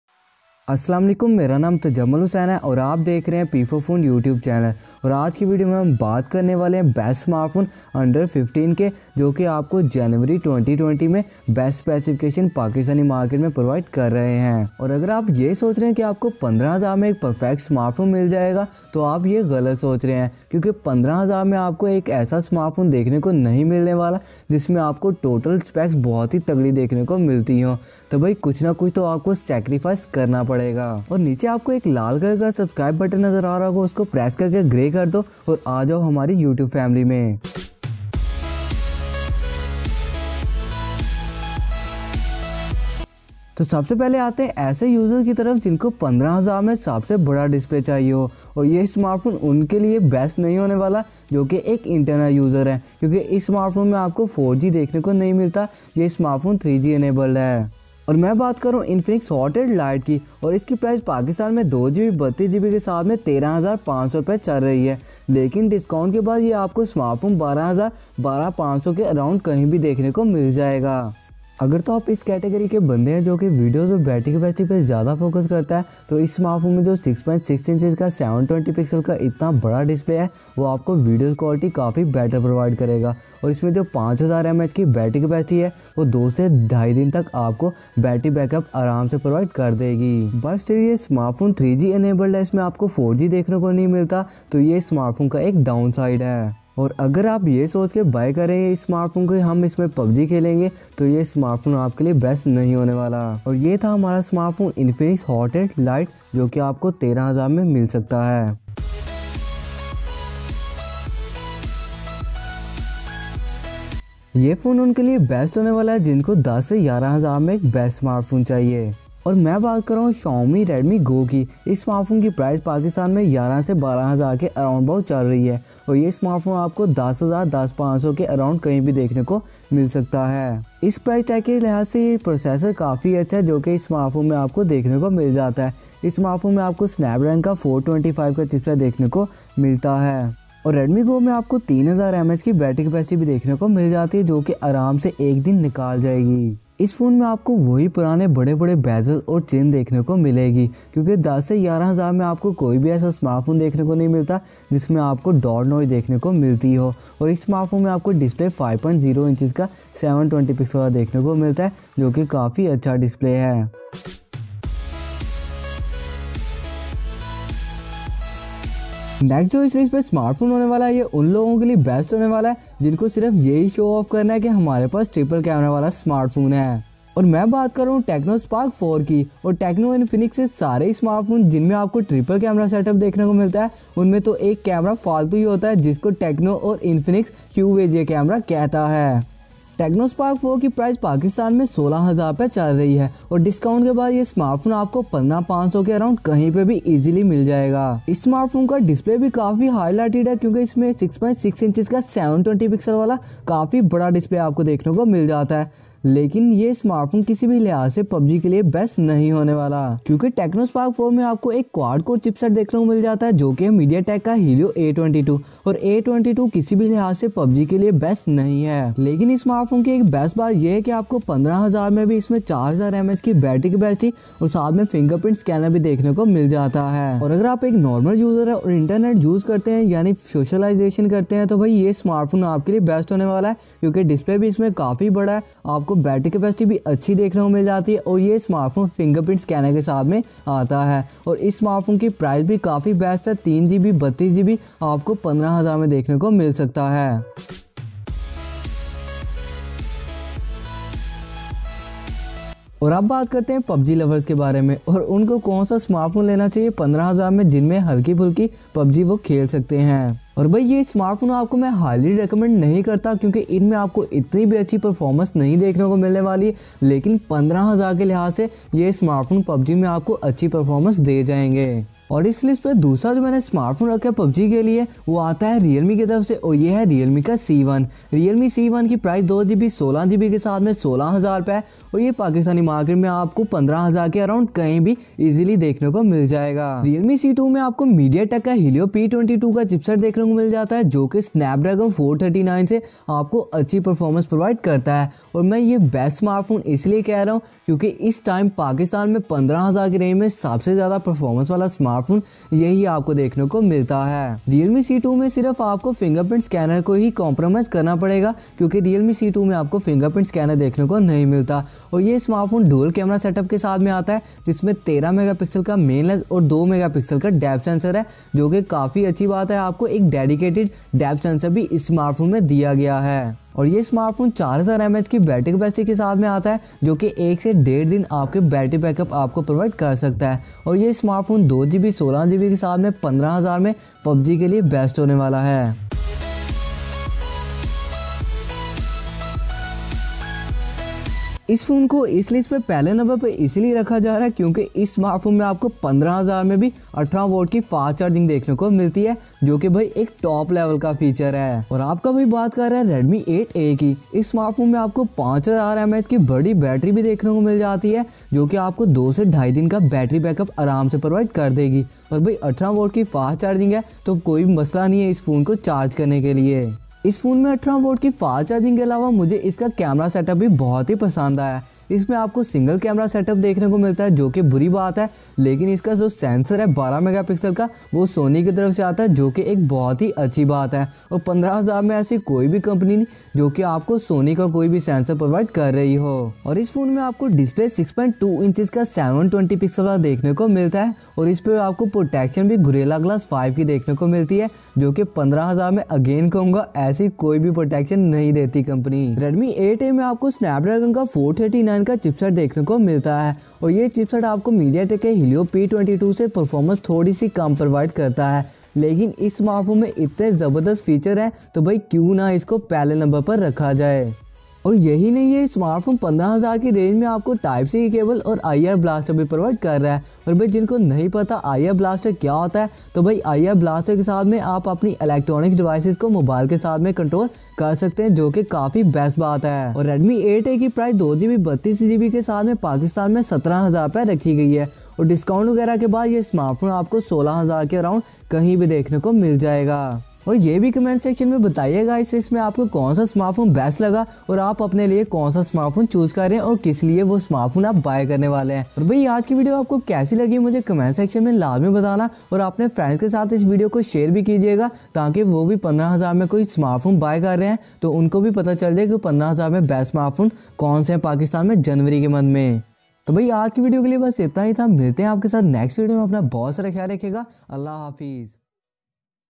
Top 5 Best Mobile Phones Under 15000 In Pakistan🔥|January 2020
Top 5 Best Mobile Phones And Smartphones Under 15000 In Pakistan / India January 2020.

Realme Best Phone Under 15000 Is Here And Infinix Or Xiaomi Best Phone Under 15000 Are included in this list.
best Smartphones Under 15000 for gaming like PUBG.